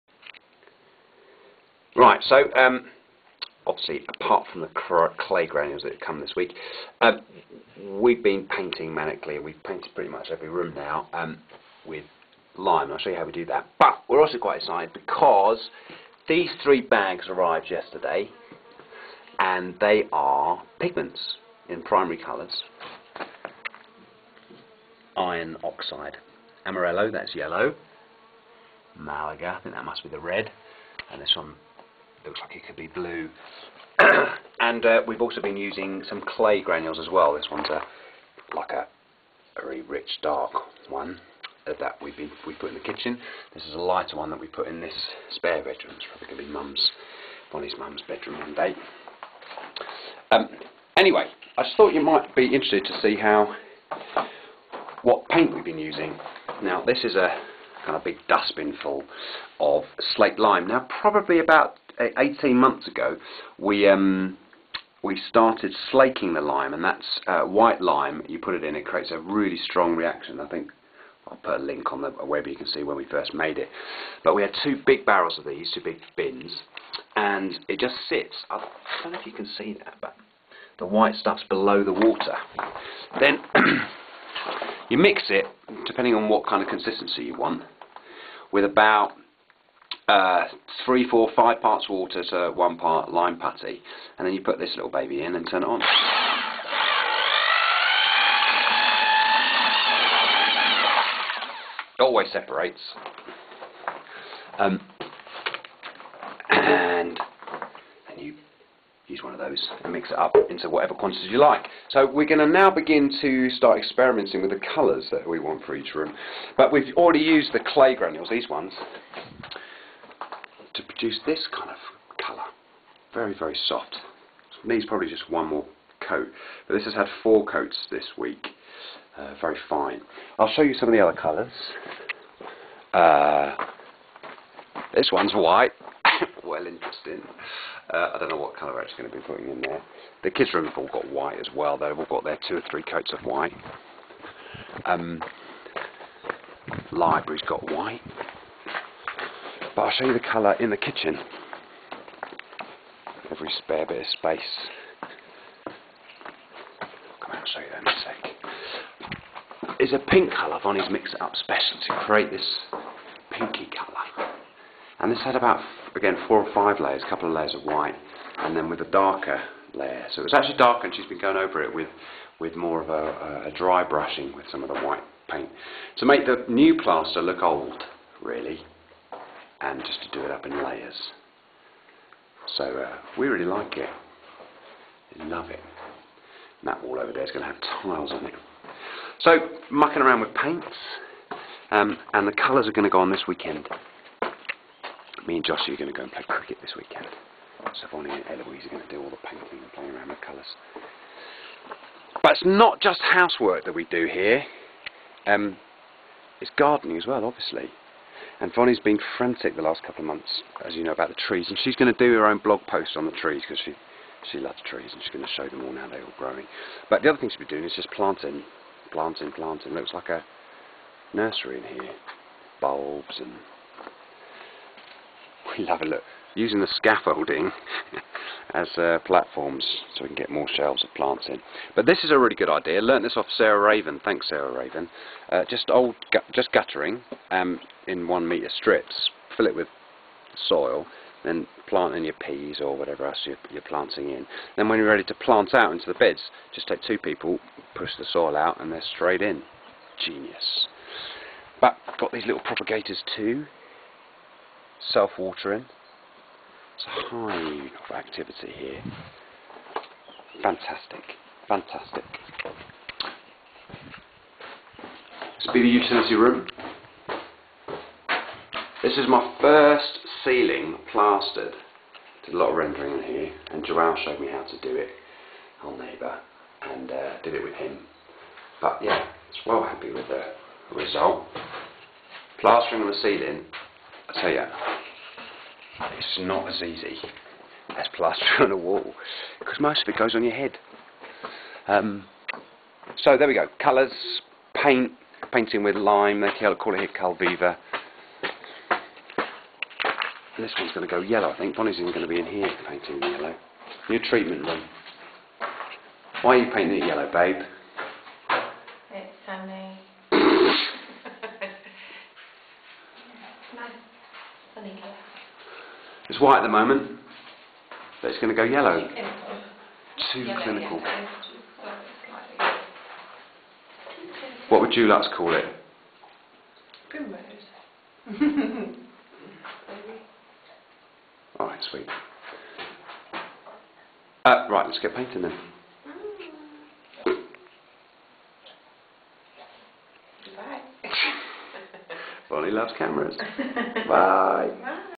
Lime Paint.MPG
Mixing slaked lime with iron oxide pigments and clay powders to make paint.